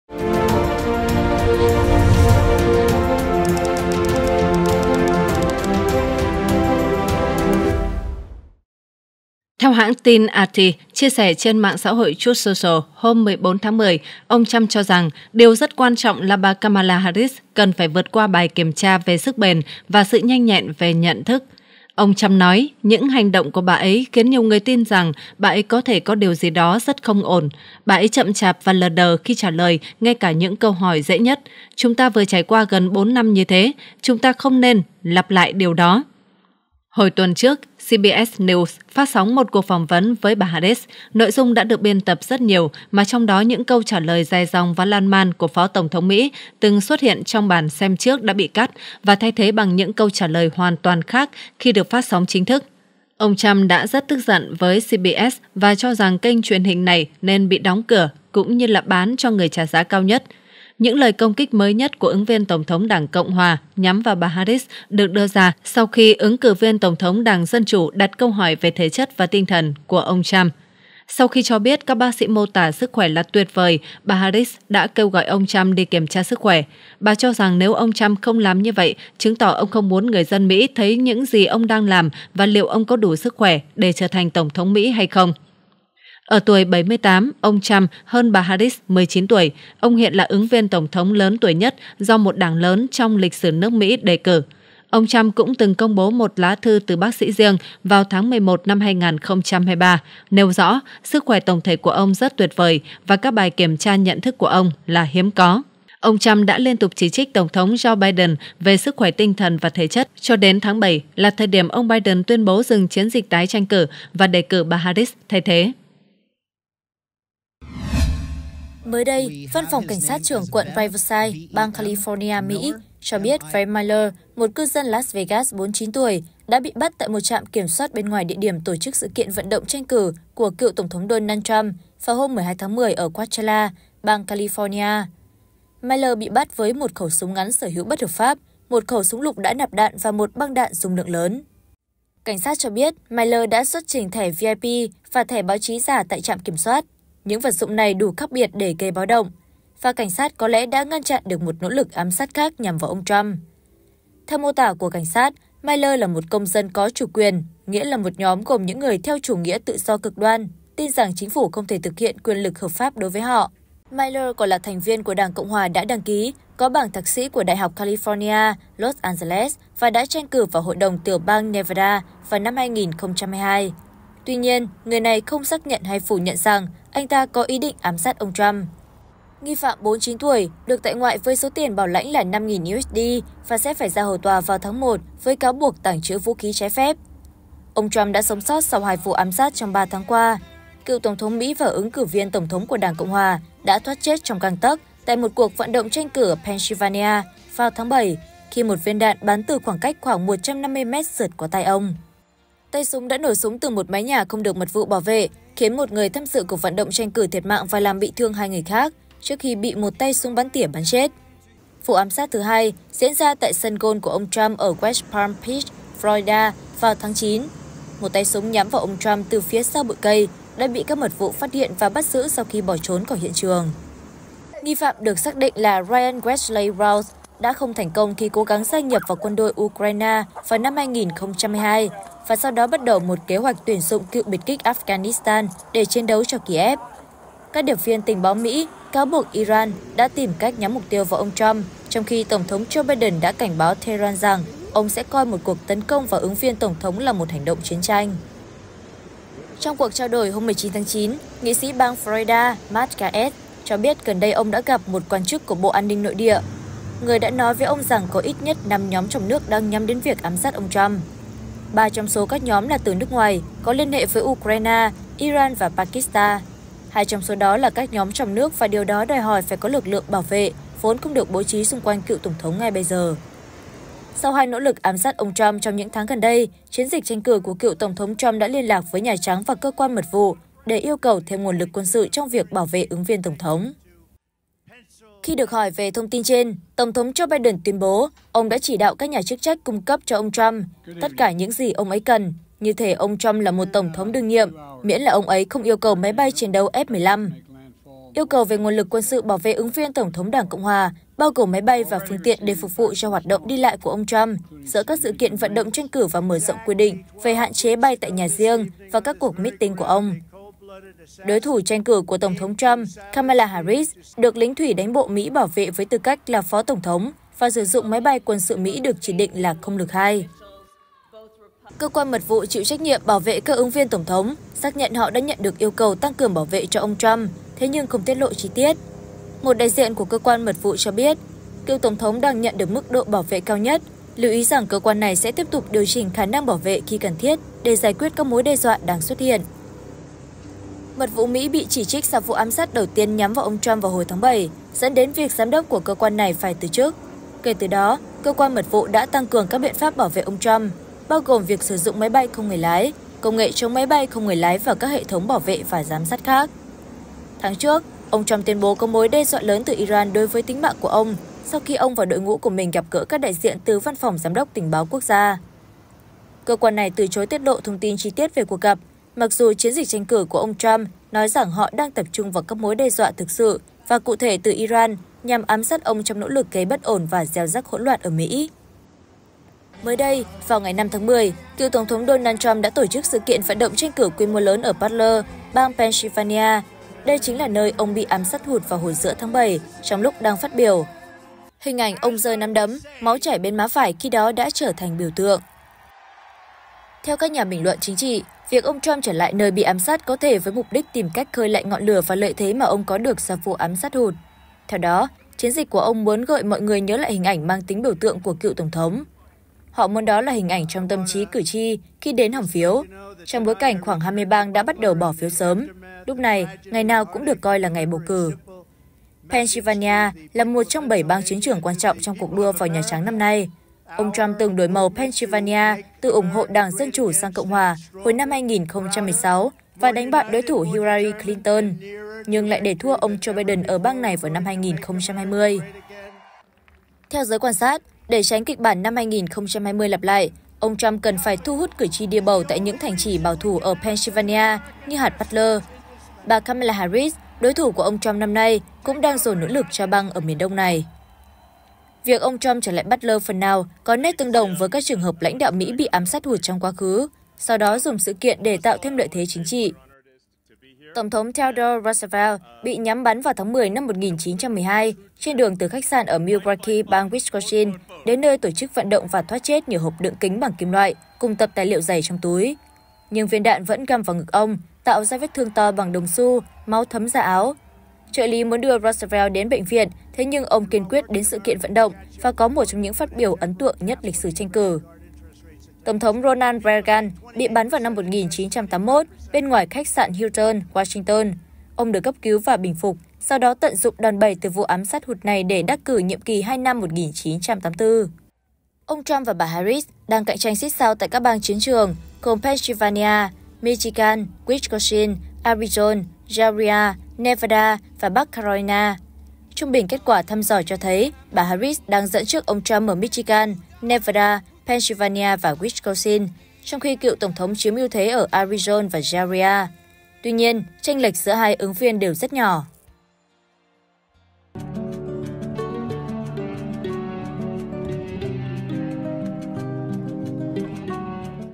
Bị thách công khai báo cáo sức khỏe, ông Trump “nổi giận” đề nghị bà Harris làm kiểm tra nhận thức?
SKĐS | Theo hãng tin RT, chia sẻ trên mạng xã hội Truth Social hôm 14/10, ông Trump cho rằng “điều rất quan trọng là bà Kamala Harris cần phải vượt qua bài kiểm tra về sức bền, và sự nhanh nhẹn về nhận thức”.